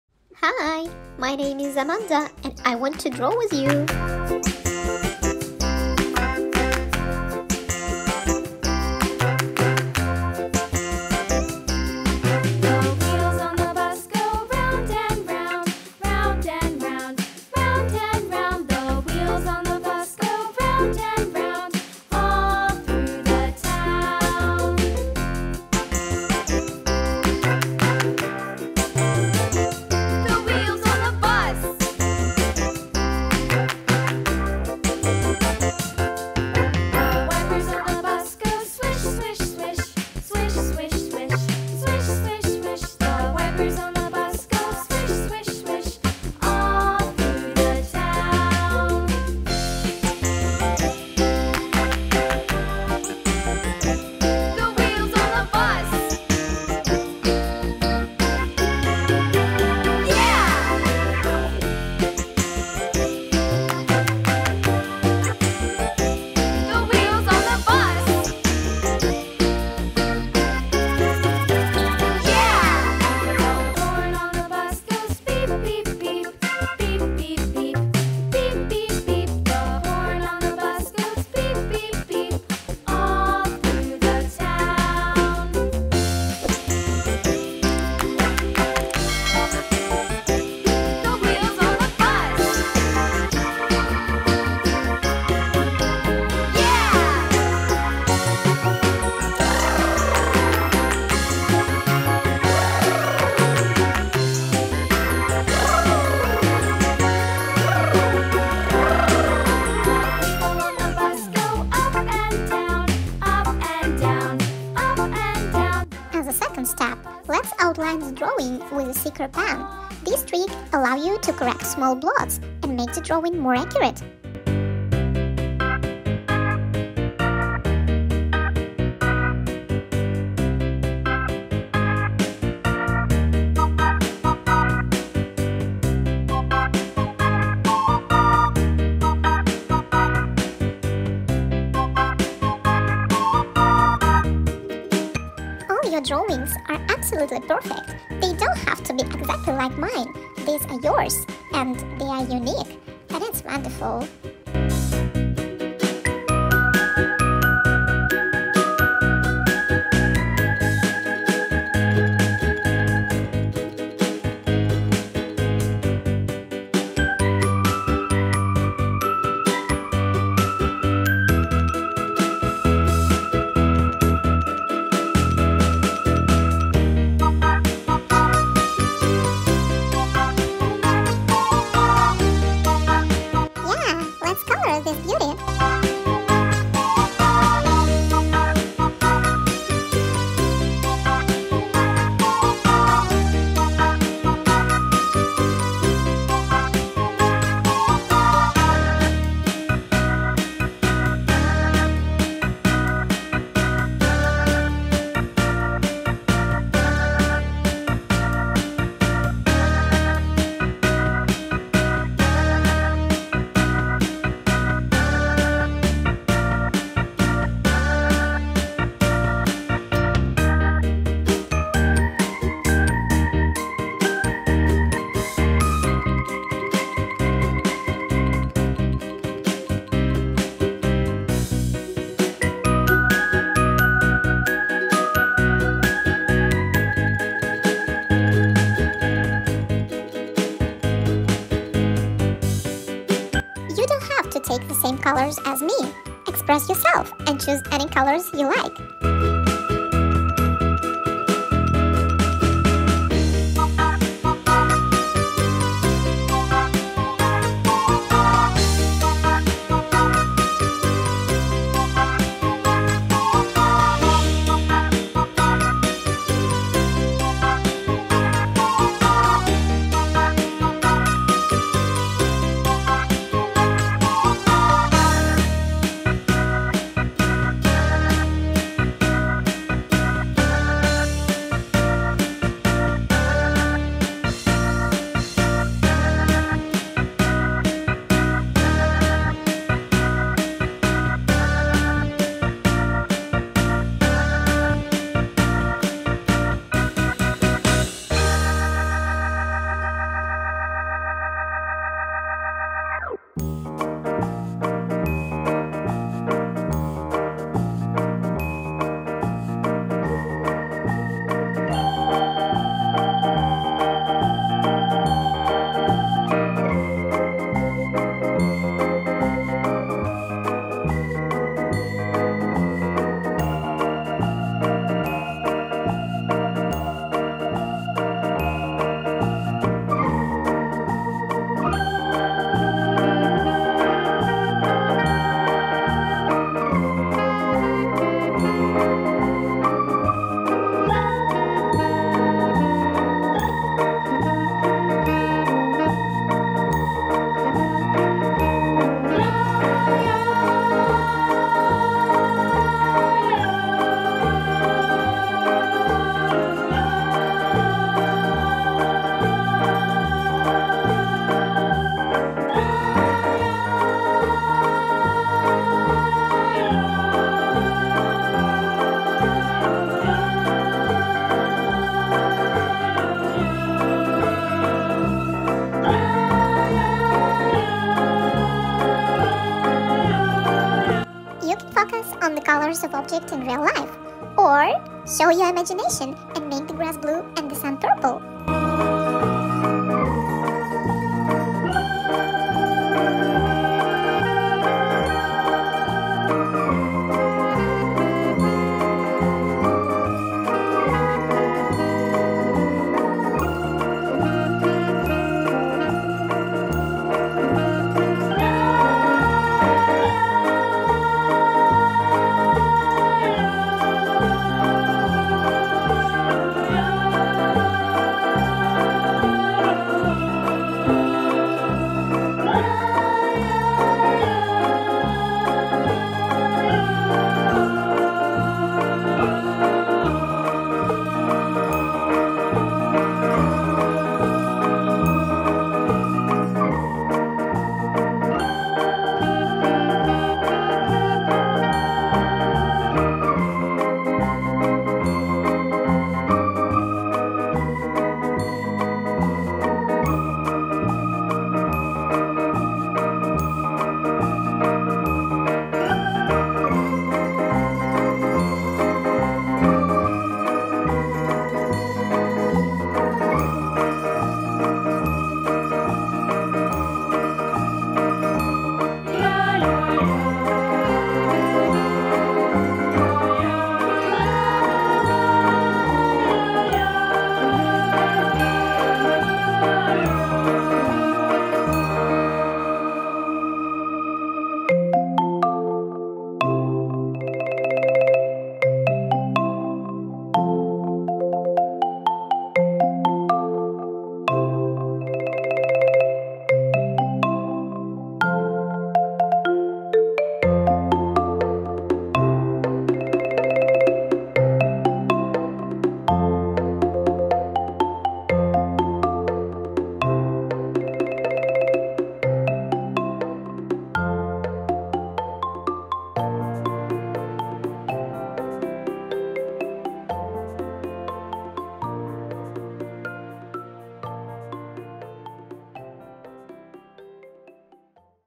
Fire Truck Drawing, Painting & Coloring For Kids and Toddlers | Child Art 🚒🔥
🚒🎨 Get ready to sound the sirens and roll out on a creative journey with our "How to Draw a Fire Truck" tutorial! Join us for an exciting adventure where little artists learn step-by-step how to bring a mighty fire truck to life. 🌟✨ Through drawing, painting, and coloring, your young heroes will capture the power and bravery of these lifesaving vehicles on paper. As they follow along, they will learn techniques to craft sturdy wheels, a vibrant red body, and all the intricate details that make fire trucks so fascinating. Let their imaginations run wild as they explore the world of firefighting and creativity.